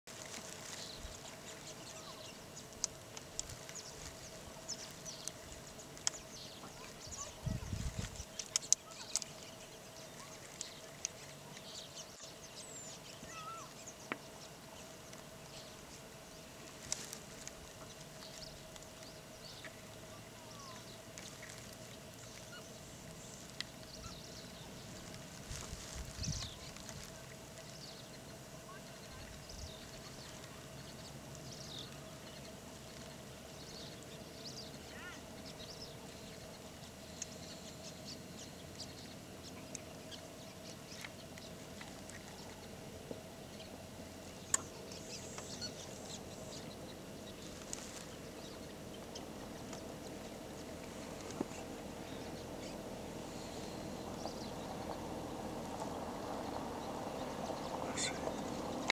KGALAGADI CHEETAH
This was taken in the Kgalagadi Transfortier park South Africa